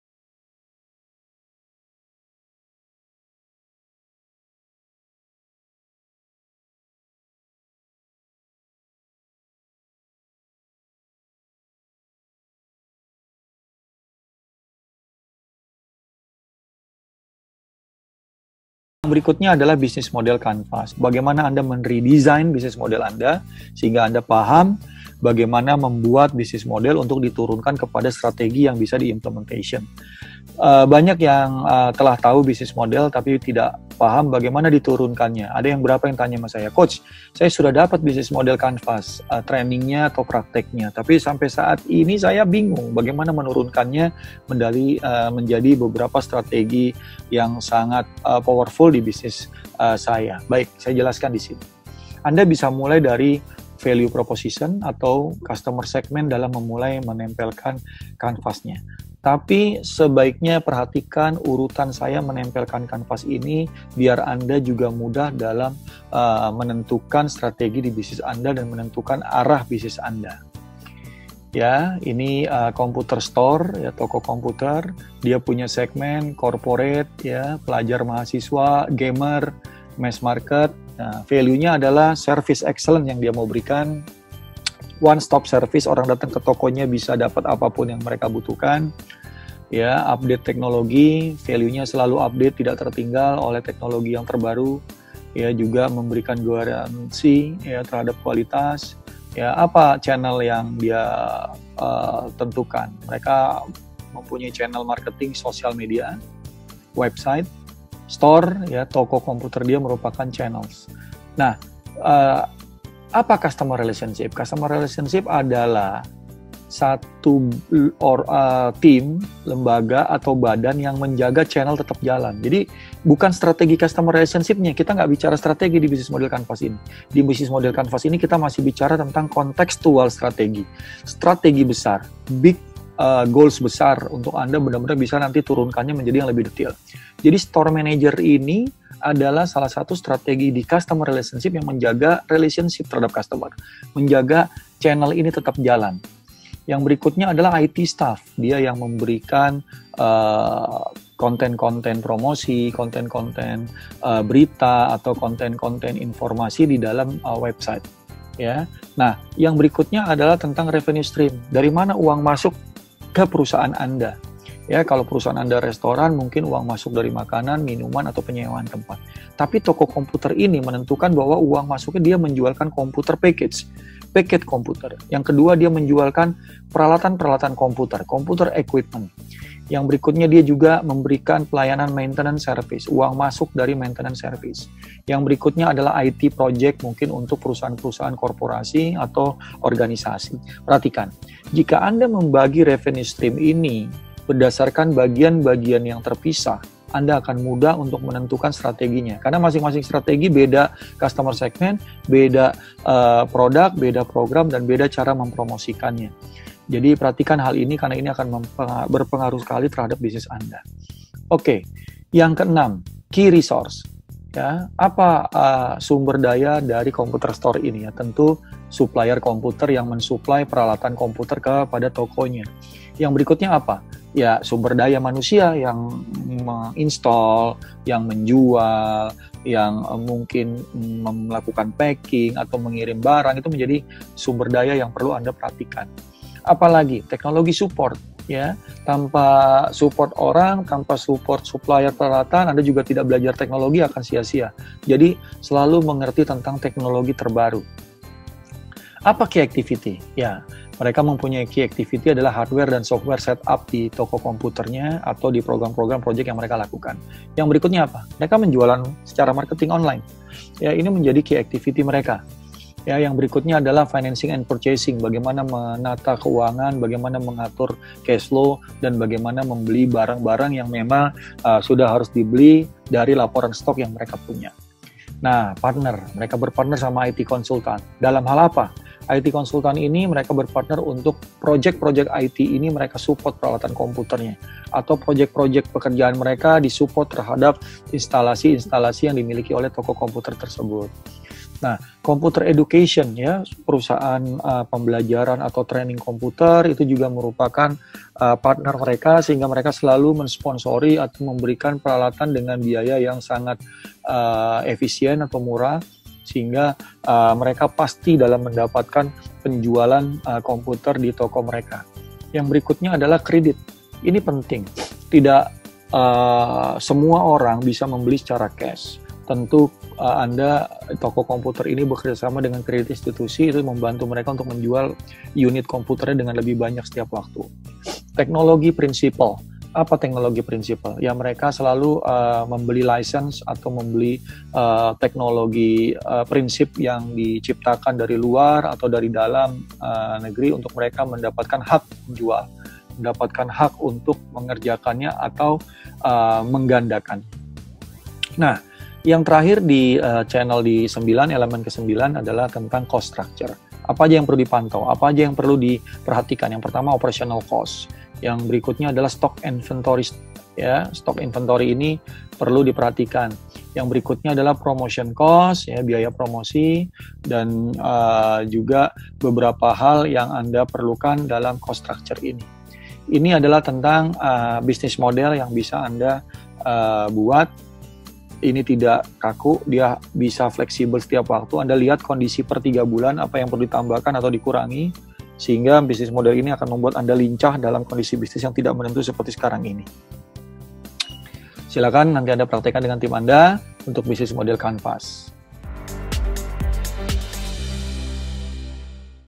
03 Business Model Canvas Strategy
Video Tutorial Tools untuk pembuatan Business Model Perusahaan Anda secara Mudah dan Terstruktur